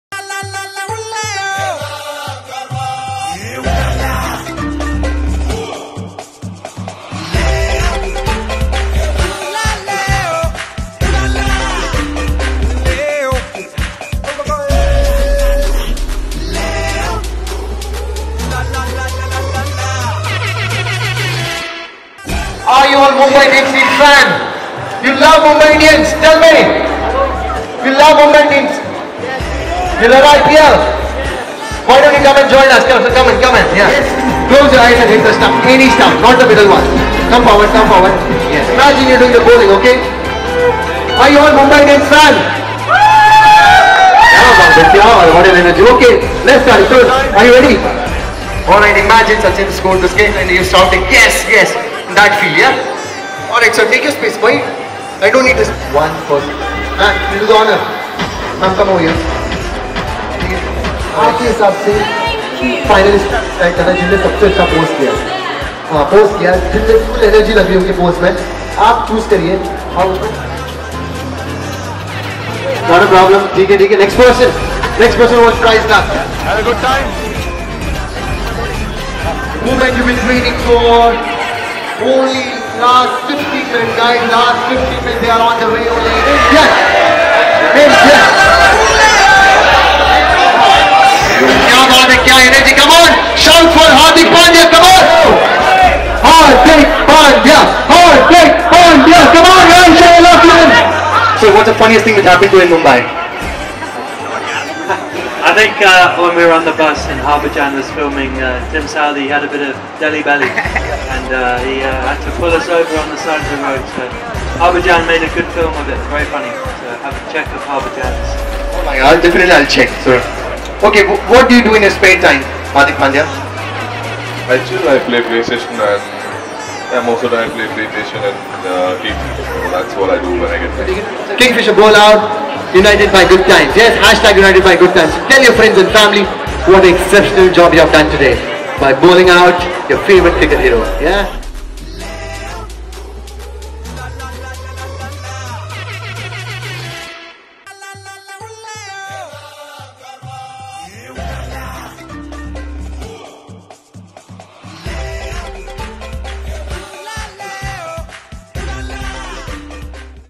Anchor Prashant Khandelwal | Kingfisher Bowl Out with the IPL Mumbai Indians Team
Anchor Prashant Khandelwal hosting the Kingfisher Bowl Out with the IPL Mumbai Indians Team